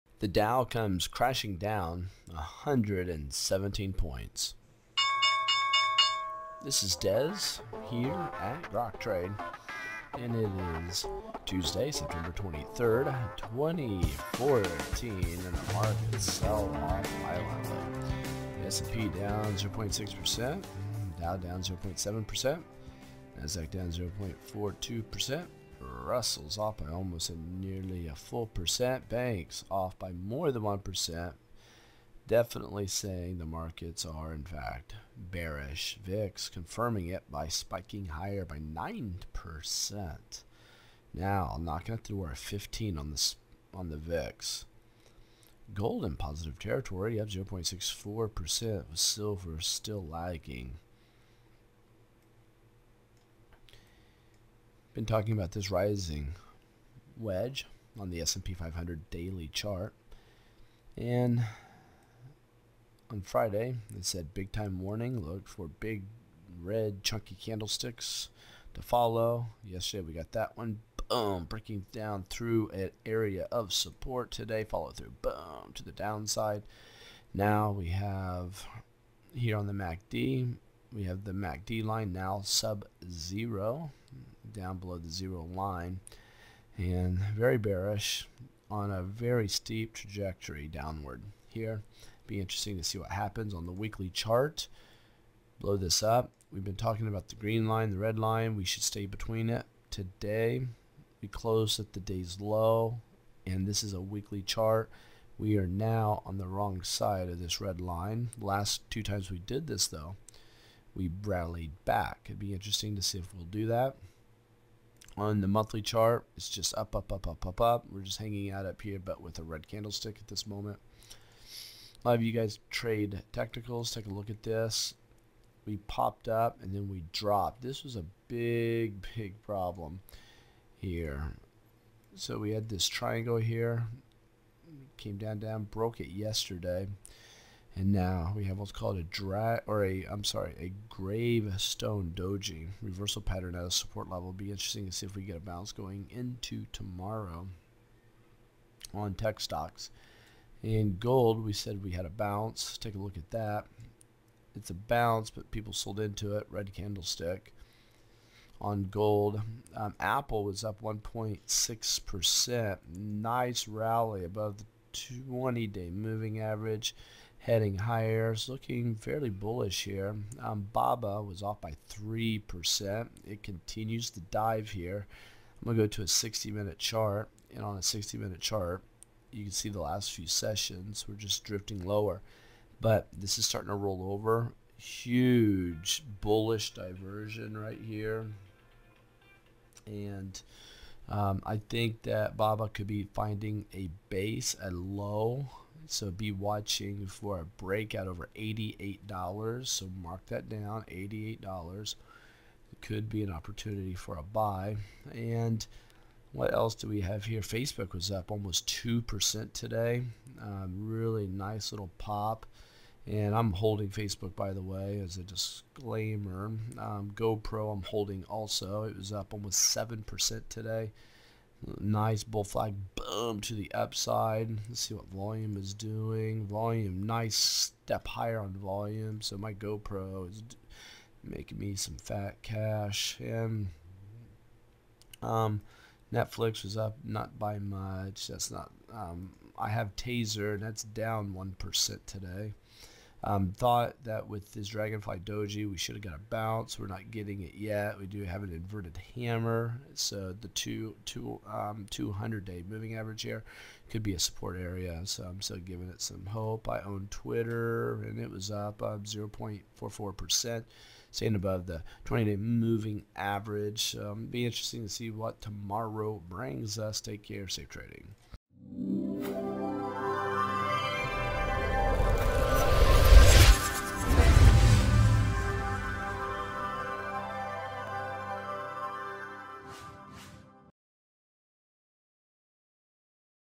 9.23.14 Stocks Continue Dive
Wall Street Continues Dive. All Major Indices Down.